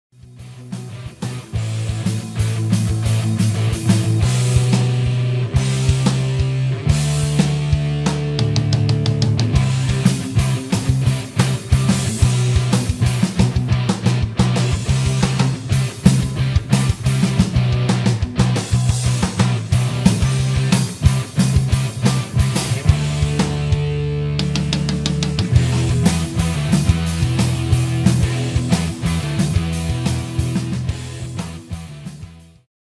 Strike 9
Started working on new songs this weekend. Here is a sample of what's to come from Project 9